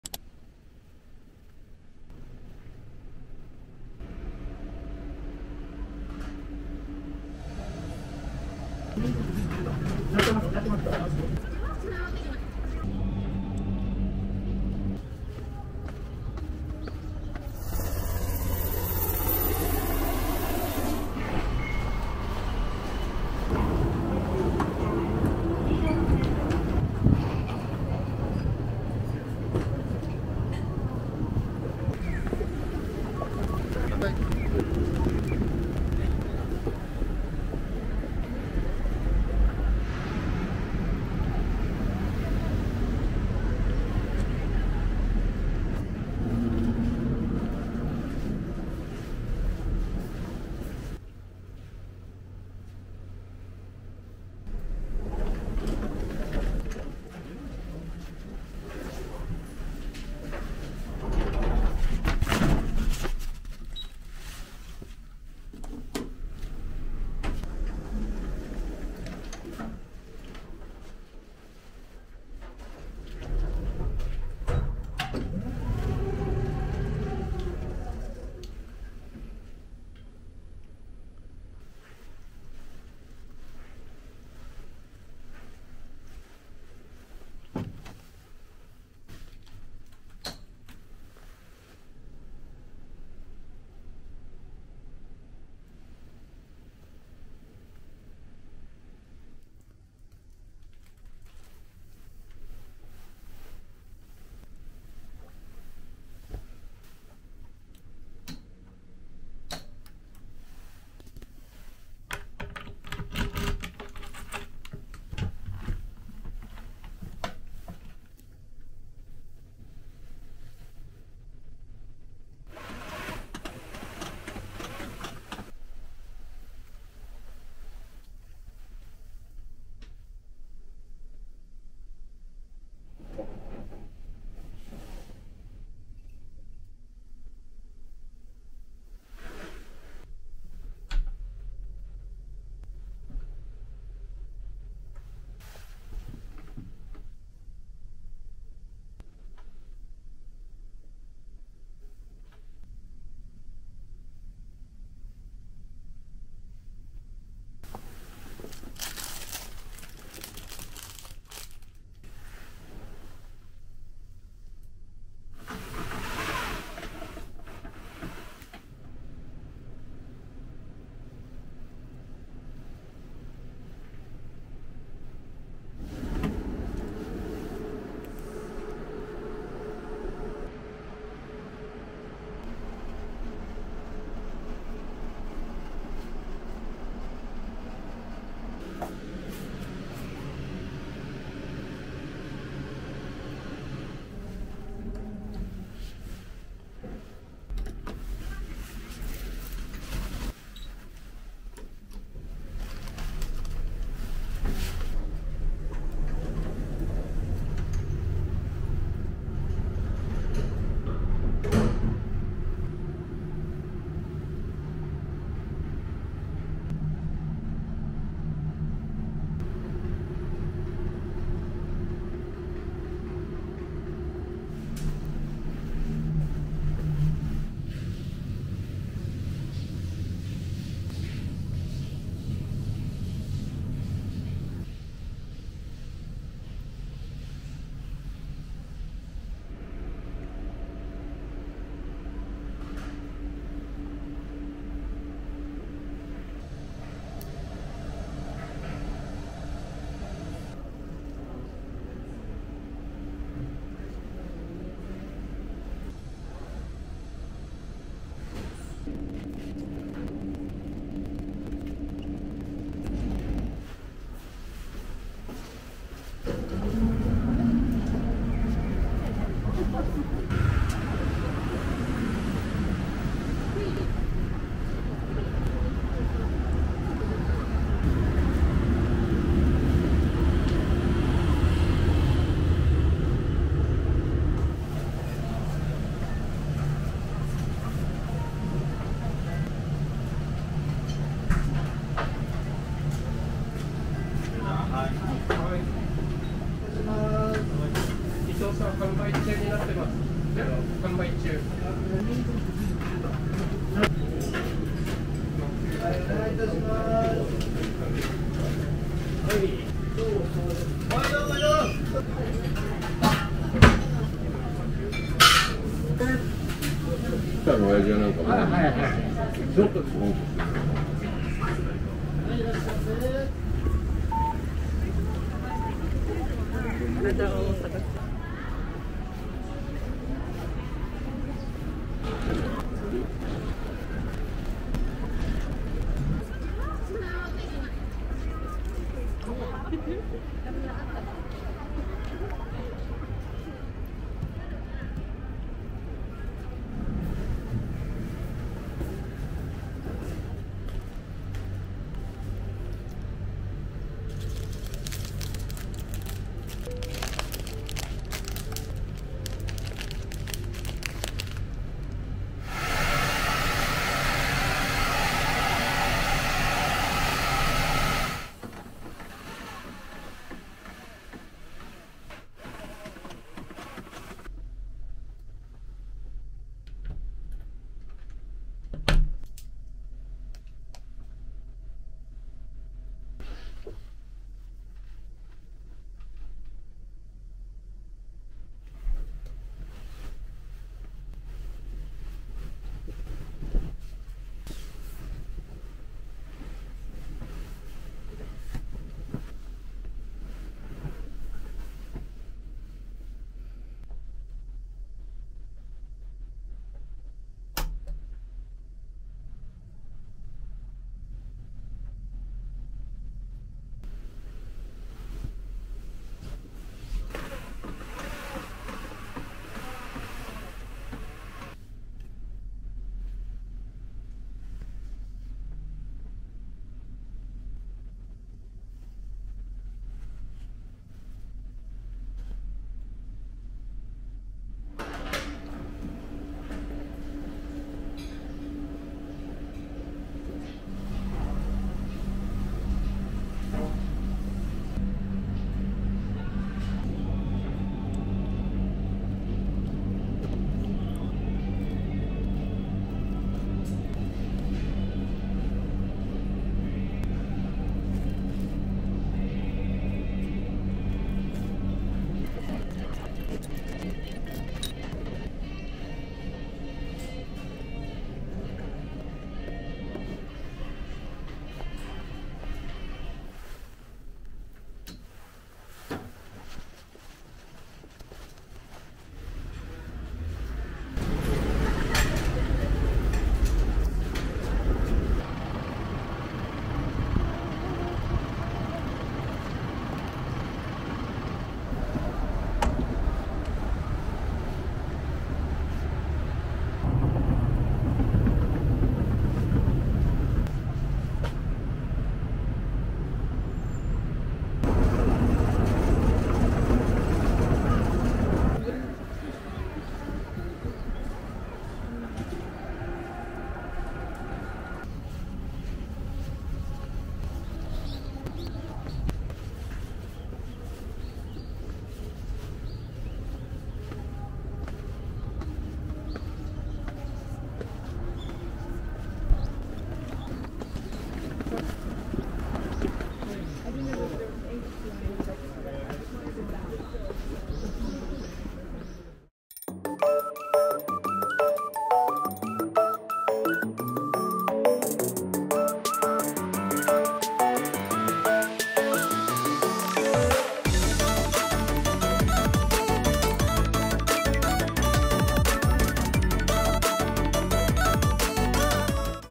Luxury capsule hotel business class was too comfortable! First Cabin Akasaka
This time I stayed at the business class cabin of "First Cabin Akasaka"!
No matter what you say, it's easy to access, and it's a 30-second walk from Akasaka Station.
There is also a large public bath and a cafe, and the rooms are so spacious that you wouldn't think it's a capsule hotel.
It was a very comfortable hotel, and I would like to use it again!

[Travel information]
Accommodation: First Cabin Akasaka
Address: 3-13-7 Akasaka, Minato-ku, Tokyo
Accommodation fee: 5,000 yen (tax included)
*Prices are subject to change, so please check the official website for details.

[If you want to travel to Japan at a great price, click here! ]
▶︎Trip.com

【chapter】
0:00 Stay at Capsule Hotel "First Cabin Akasaka"
01:32 Capsule room introduction
02:56 washbasin, toilet
03:25 Public bath, lounge, cafe
04:23 Dinner, walk
05:47 Bed preparation
06:49 Breakfast, check out
08:11 Hie Shrine